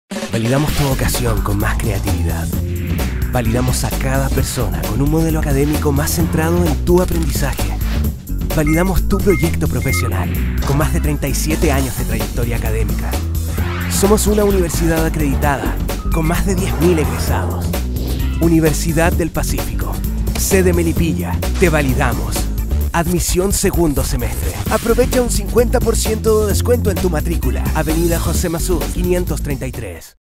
Campaña Admisión 2 Semestre 2013 - Universidad del Pacífico, Creatividad para Emprender
Campaña Admisión 2° Semestre 2013, Universidad del Pacífico, Creatividad para Emprender (Sede Melipilla)

Postula a la universidad en el siguiente y se te asignara un ejecutivo de admisión quien te orientará para aclarar todas tus dudas.

También puedes visitar la página de la universidad para conocer la oferta de carreras que la universidad tiene para ti.